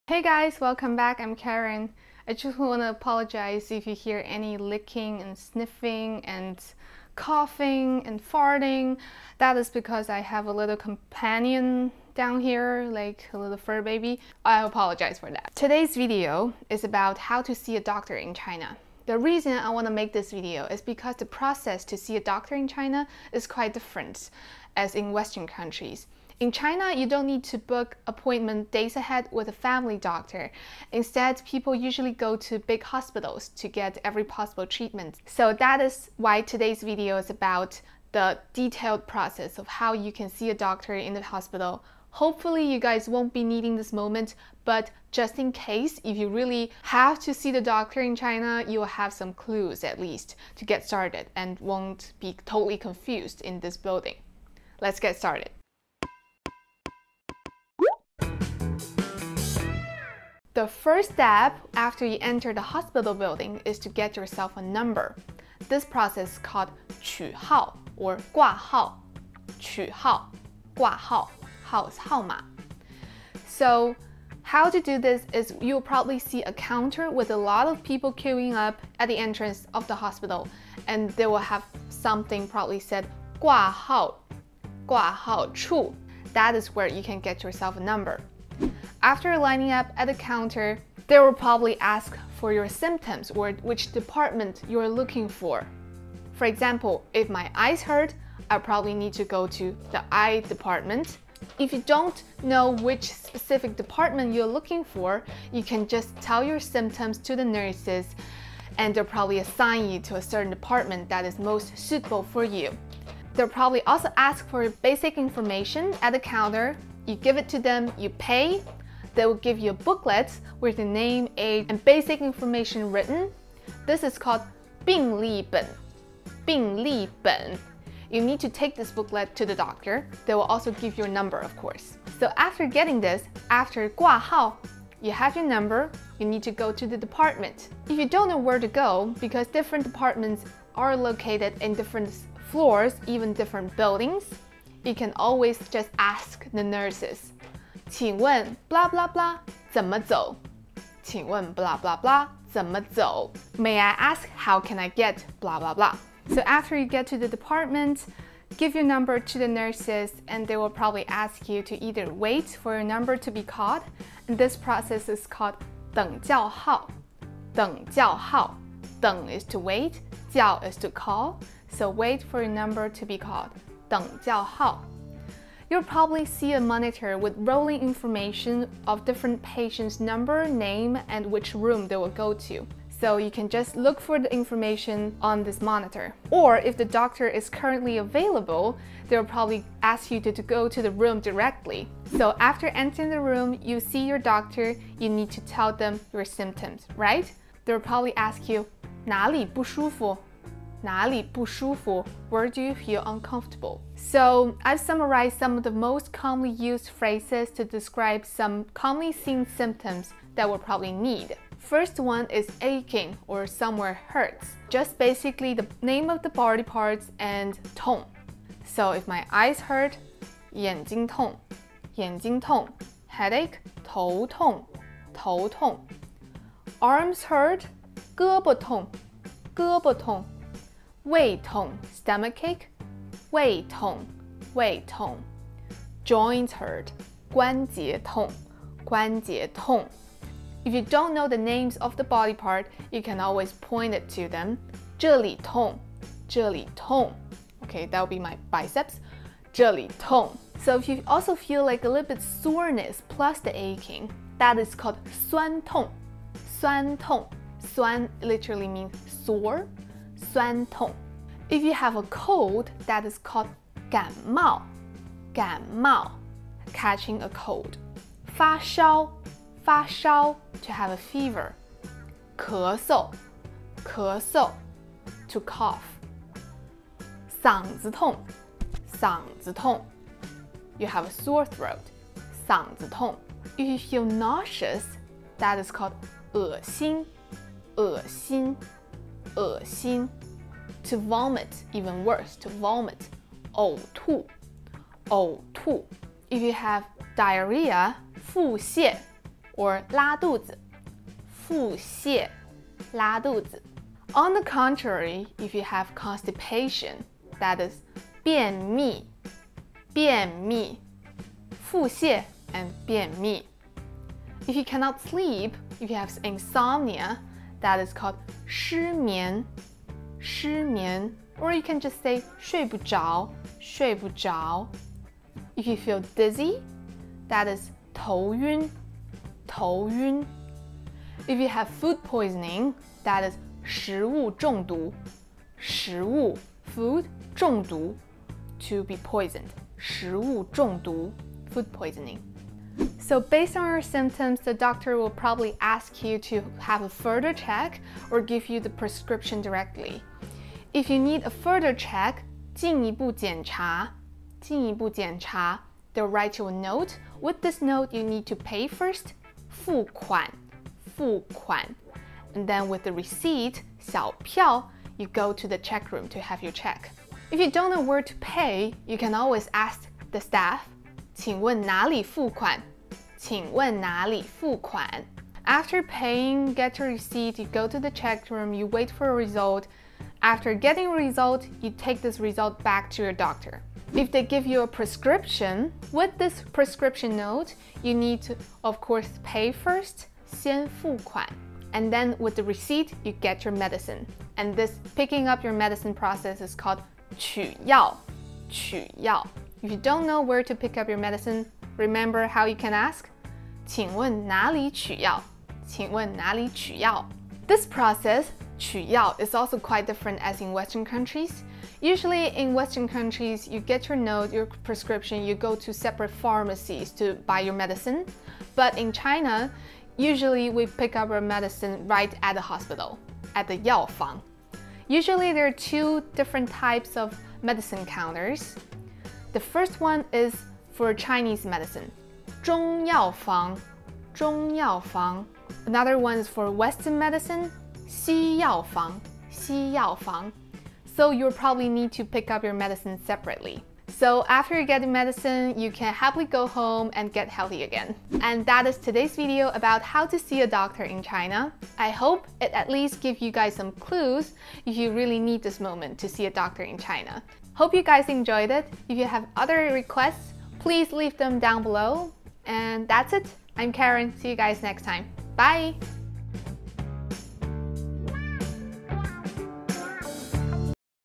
How to SEE A DOCTOR in China | Chilling Chinese | Survival Chinese Vocabulary
Hey guys, welcome back to Chilling Chinese!

Today’s video is about how to see a doctor in China!
As you guys may or may not know,
Visiting a doctor in China is a completely different experience!

Hopefully after watching this video, you guys will have some clues when this moment comes!

Words and Sentences Mentioned:
取号/挂号 （qǔ hào/guà hào）
号码 （hào mǎ）
挂号处 （guà hào chù）
病历（本）（bìng lì běn）
等叫号 （děng jiào hào）
看病 （kàn bìng）
哪里不舒服？（nǎlǐ bù shūfu）
眼睛痛 （yǎnjīng tòng）
头痛 （tóu tòng）
胳膊痛 （gēbo tòng）
胃痛 （wèi tòng）
关节痛（guānjié tòng）
酸痛（suān tòng）
感冒 （gǎn mào）
发烧 （fā shāo）
咳嗽 （ké sou）
嗓子痛 （sǎngzi tòng）
恶心 （ě xin）
呕吐 （ǒu tù）
腹泻/拉肚子 （fù xiè / lā dùzi）
便秘 （biàn mì）
失眠/睡不着 （shī mián / shuì bù zháo）
头晕 （tóu yūn）
食物中毒 （shíwù zhòngdú）
进一步检查 （jìn yí bù jiǎnchá）
付款 （fù kuǎn）
小票 （xiǎo piào）
请问哪里付款？（qǐngwèn nǎlǐ fùkuǎn）
请问哪里取药？（qǐngwèn nǎlǐ qǔyào）
取药 （qǔ yào）
药房 （yào fáng）
中药方 （zhōng yào fáng）
西药房 （xī yào fáng）
请问。。怎么走？（qǐngwèn zěnme zǒu）

图片来源于网络

Check out our Instagram to see DAILY posts where you can get some extra chilling learning!